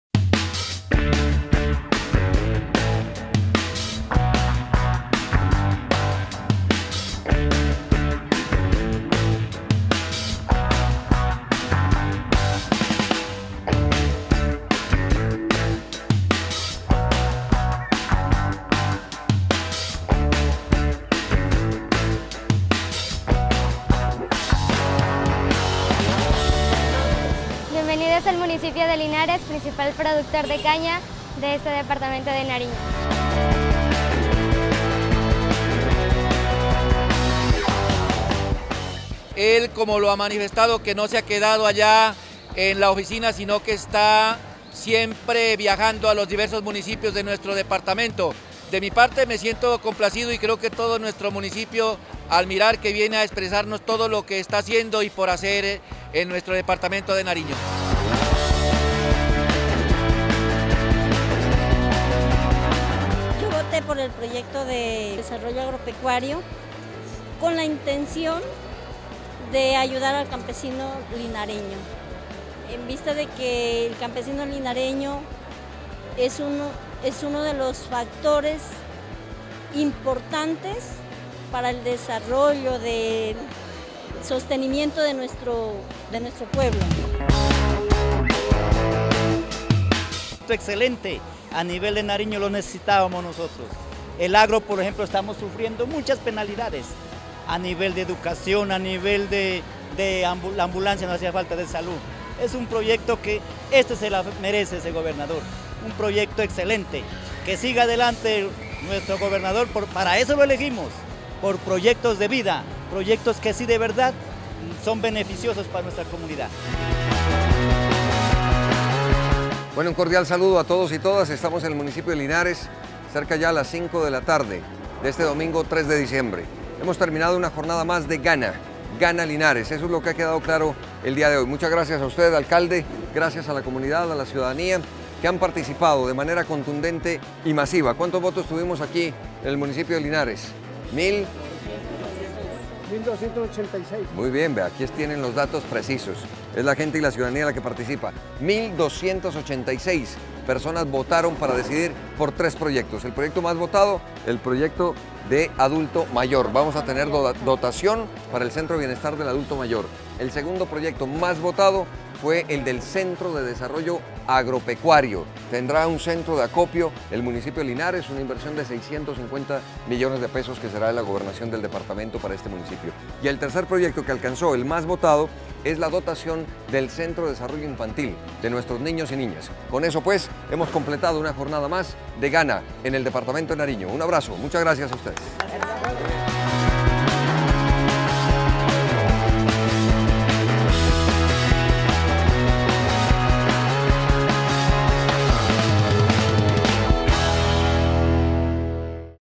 Así se vivió GANA Linares
La construcción de un Centro de Desarrollo Agropecuario, así como la dotación para el Centro de Bienestar del Adulto Mayor y el Centro de Desarrollo Infantil fueron los proyectos elegidos por el pueblo linareño en la jornada del GANA municipal en esta zona del suroccidente nariñense.